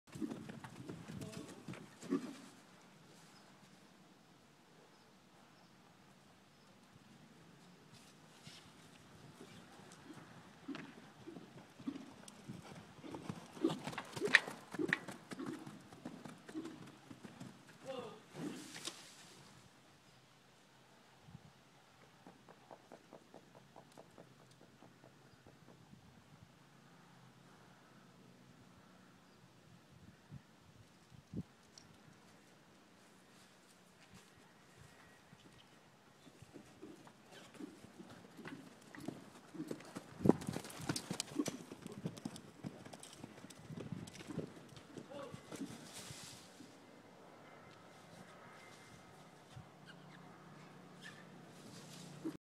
R better stops
teaching "raceable" how to stop on command using seat and vocal cues. When he stops without pressure from the bit he gets praise and a 5 second rest. if he does not stop well, then he has to turn or reverse. (The last stop in the video he chose to back himself, I didn't ask him to so I loped him forward.)